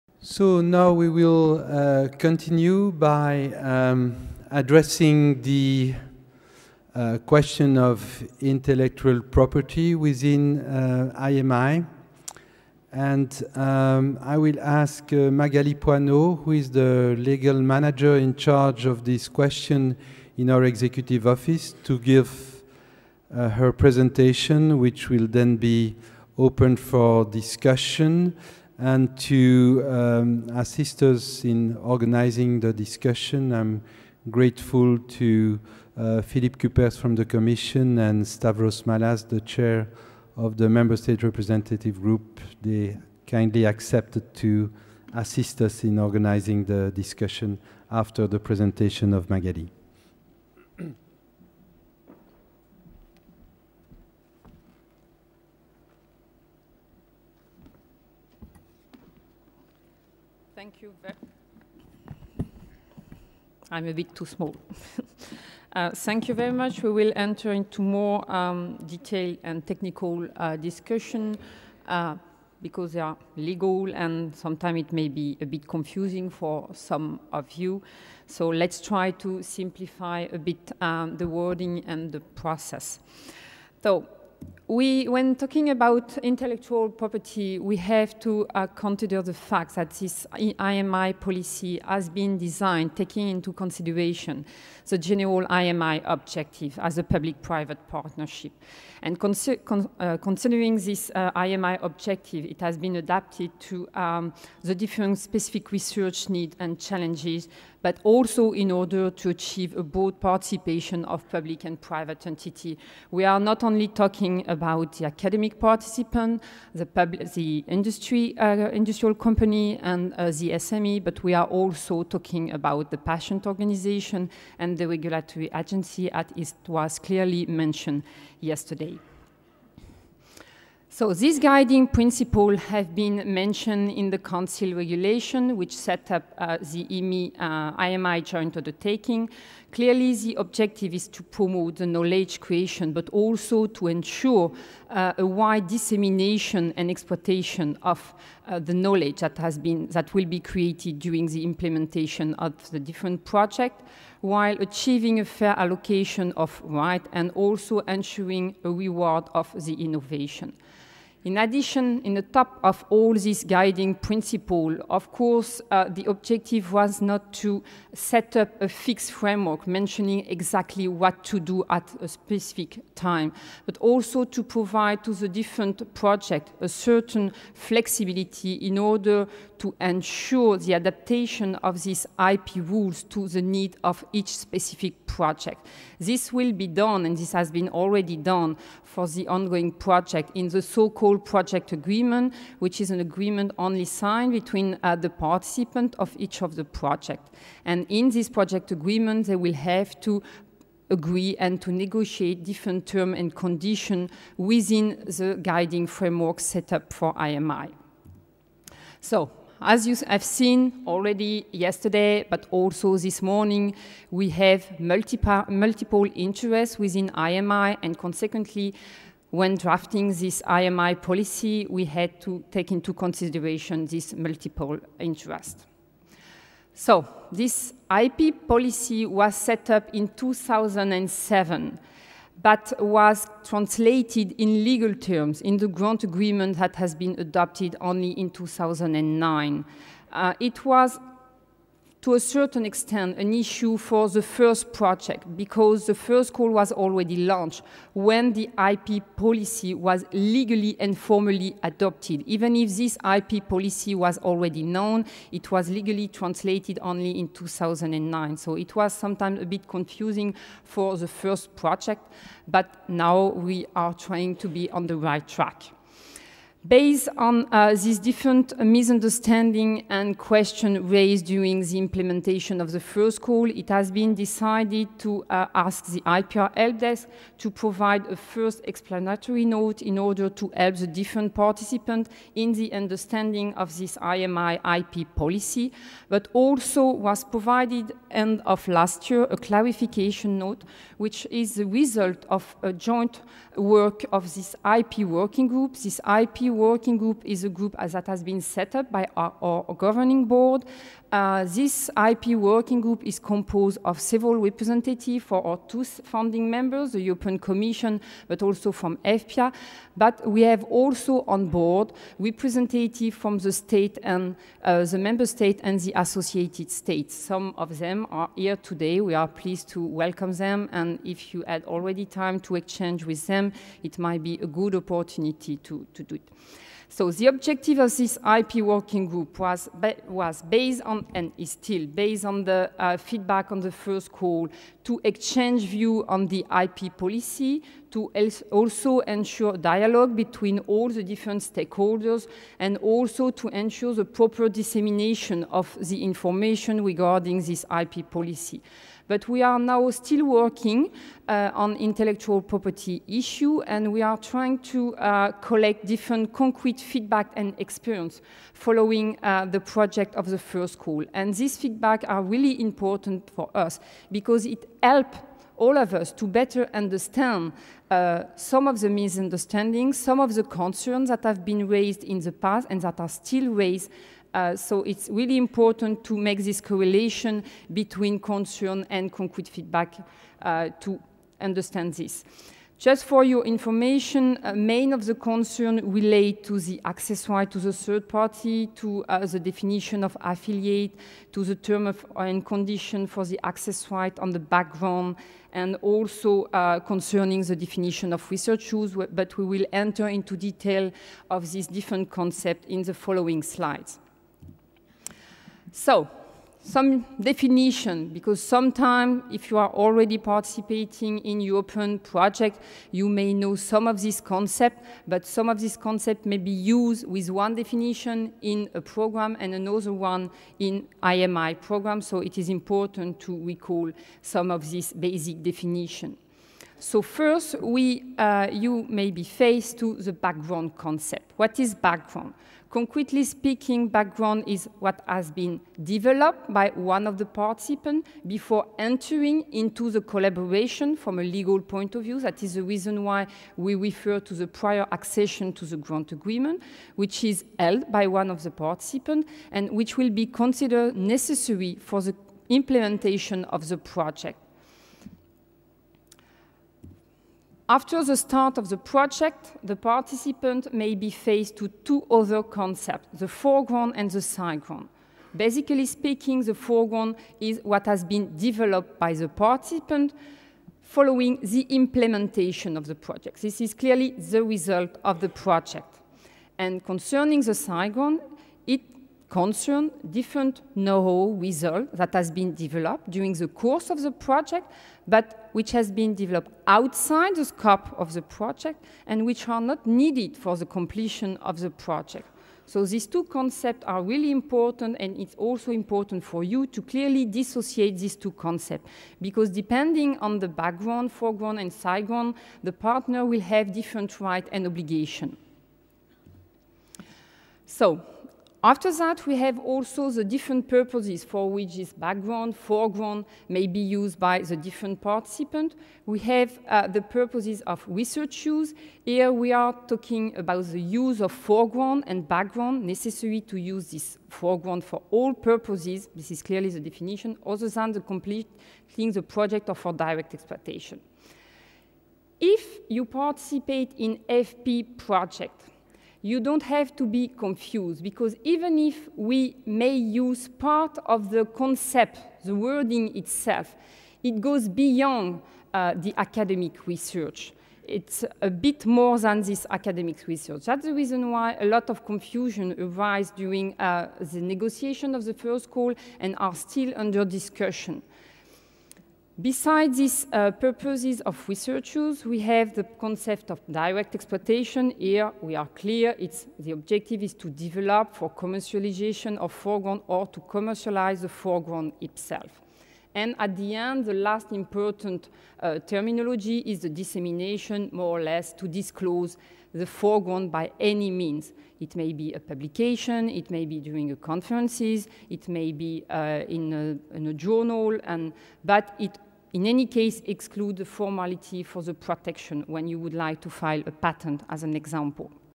10) Magali Poinot: New Ways of Sharing Knowledge (Part 1/3)
Magali Poinot, Legal Manager IMI
Stakeholderforum 2010, 14-15 June 2010
www. imi.europa.eu